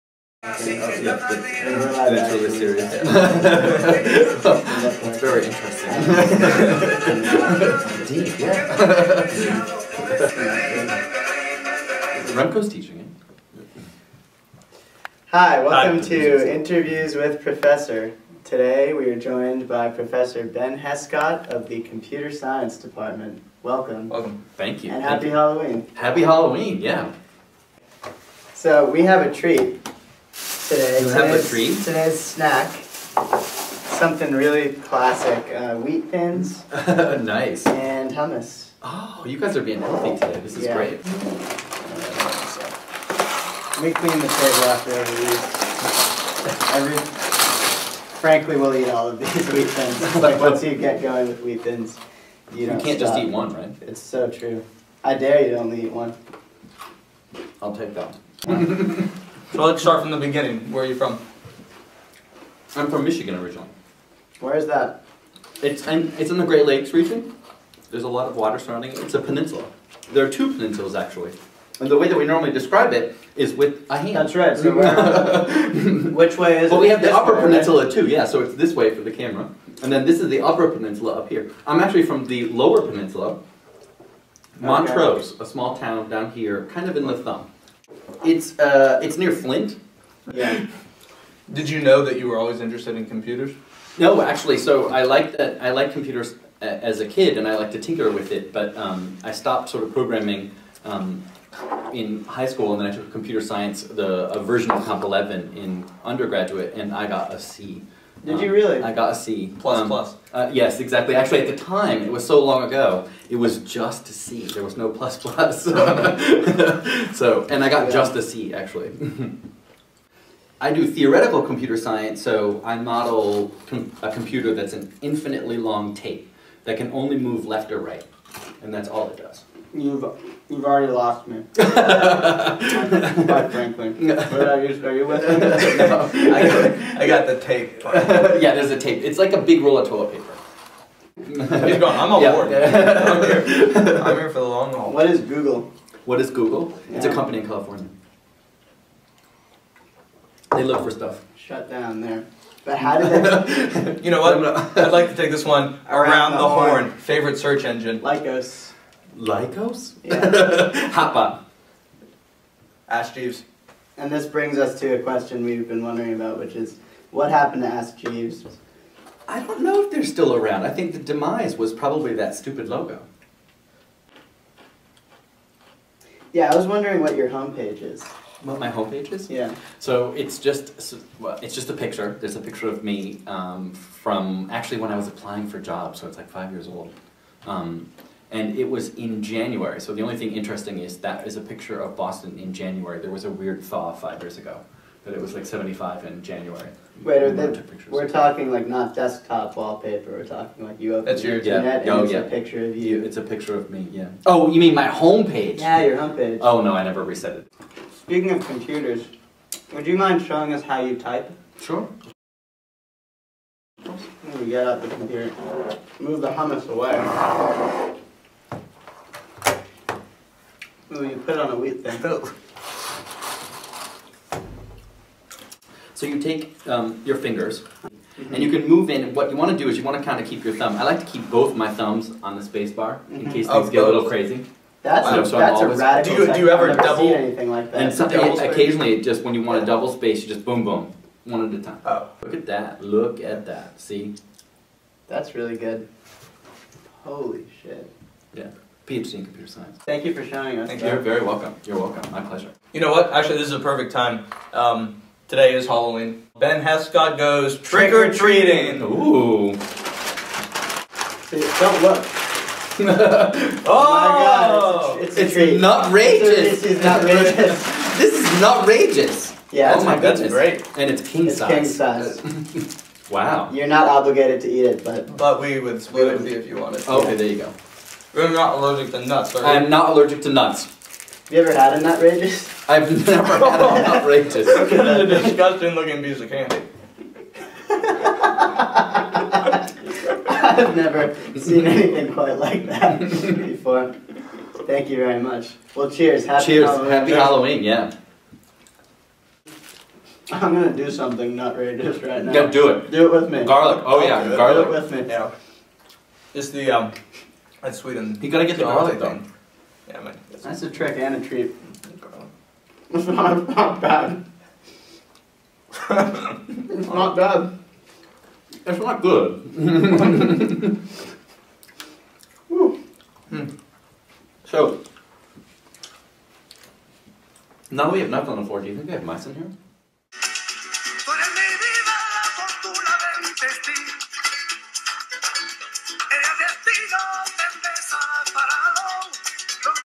"Interviews with Professor" Ep. 3: Ben Hescott
Danny and Luke take on Tufts computer science professor Ben Hescott in the third installment of "Interviews with Professor."